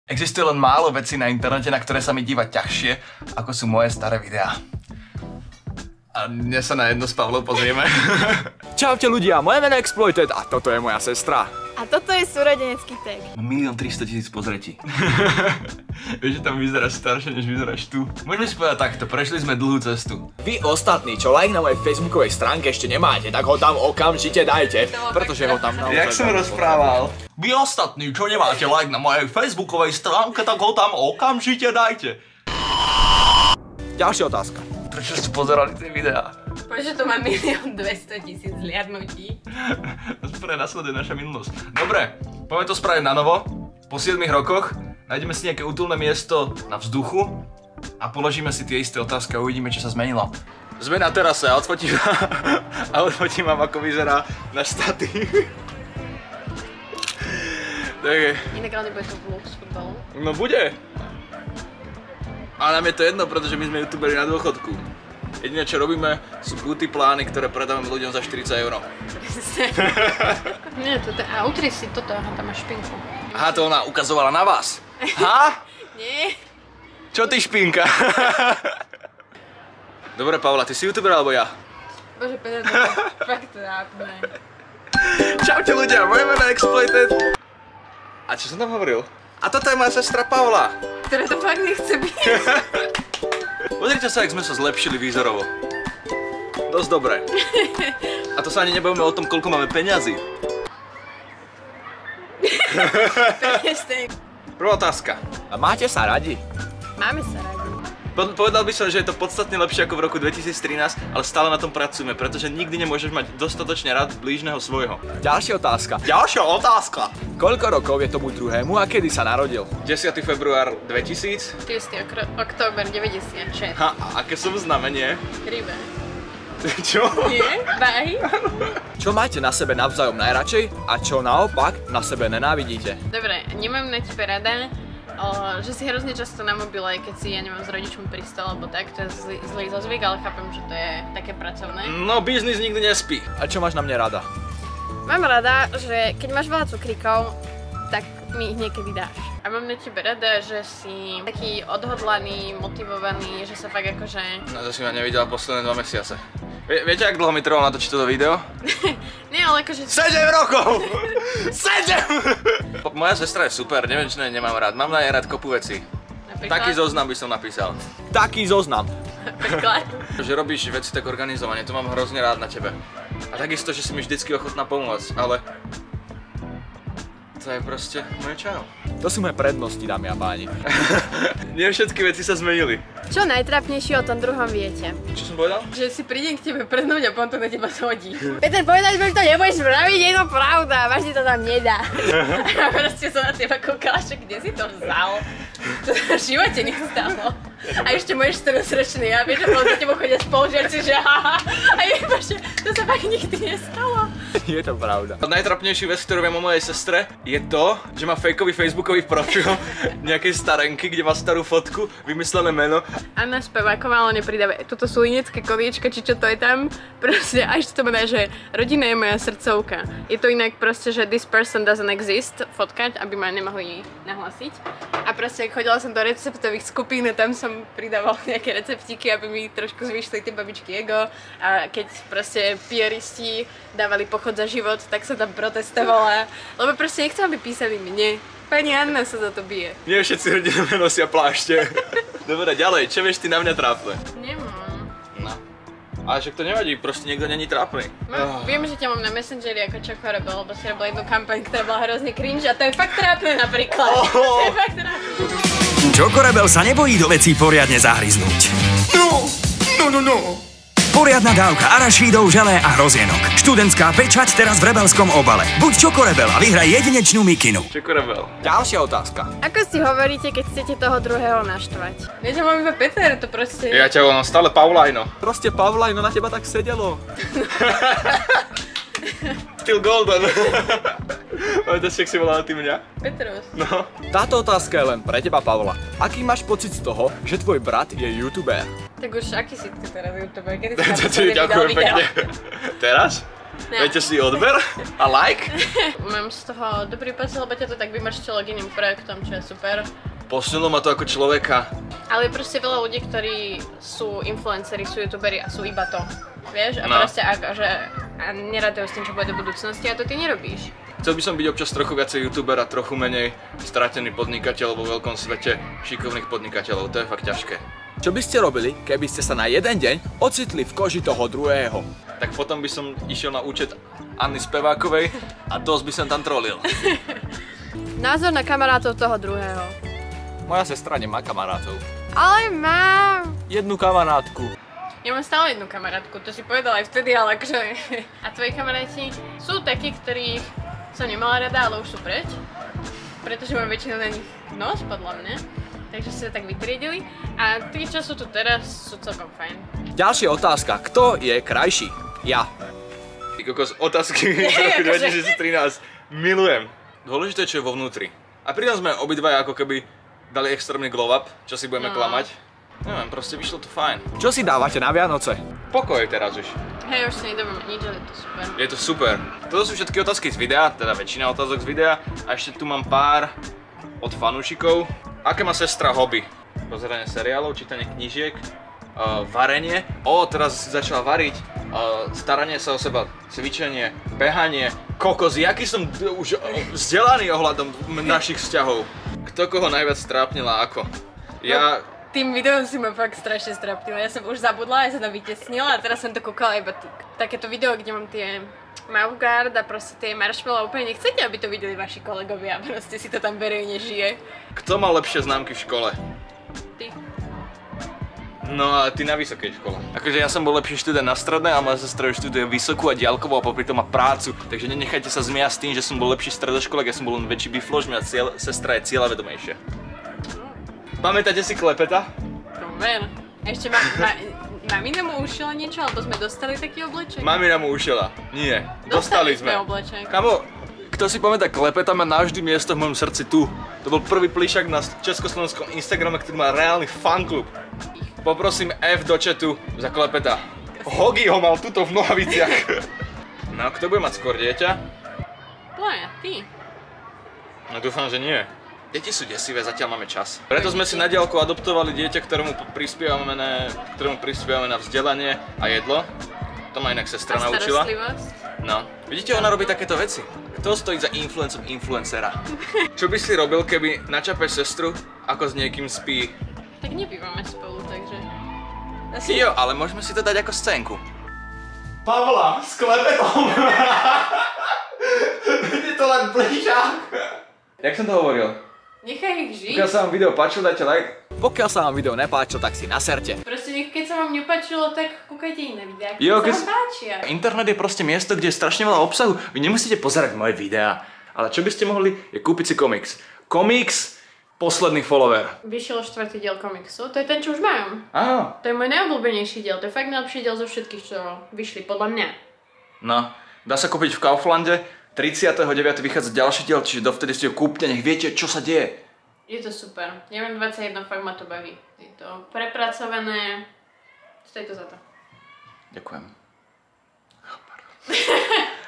PO 7. ROKOCH !!!
PO 7 ROKOCH sme sa pozreli na naše staré video. A bolo to náročné :D ! Ale sranda !